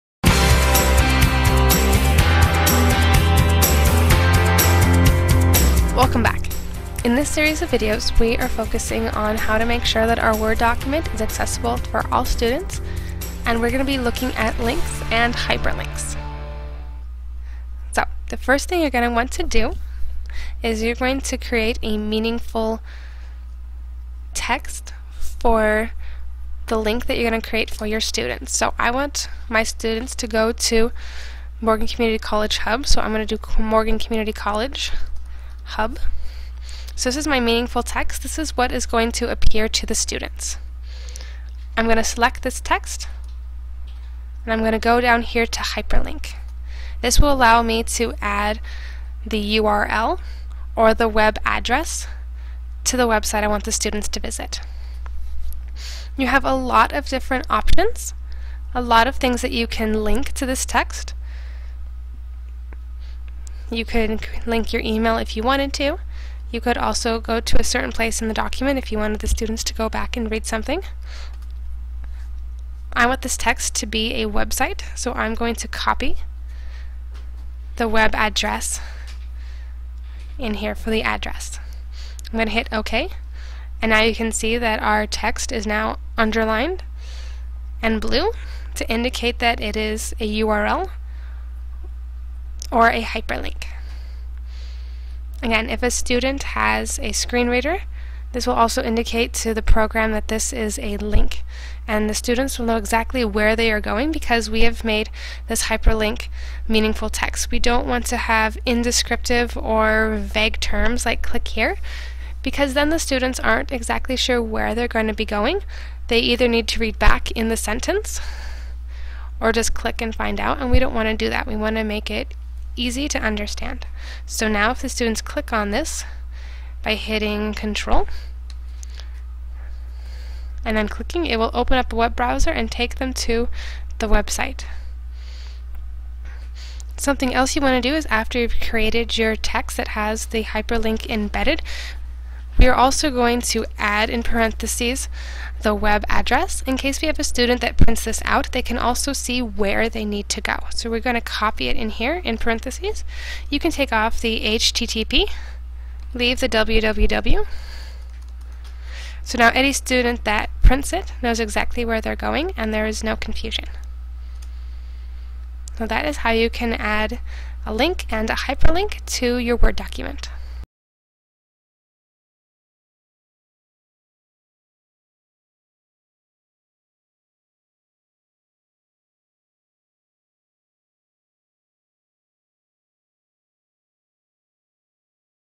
5 Links in Microsoft Word
Links are a major method of navigating for everyone, but especially screen reader users. If the links are embedded into meaningful text, they are much more useful.